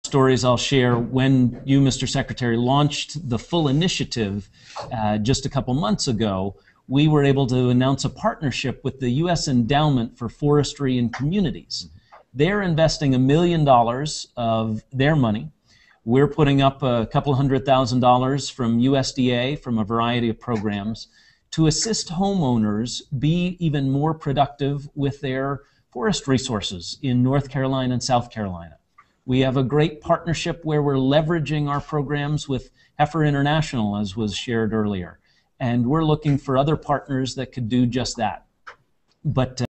Agricutlure Secretary cites Endowment Partnership
In a Google+ Hangout to discuss opportunities available through USDA's StrikeForce Initiative for Rural Growth and Opportunity, Agriculture Secretary Tom Vilsack and StrikeForce Initiative Coordinator Max Finberg cite the Endowment-led Sustainable Forestry and African American Land Retention Program as an example of exemplary partnership.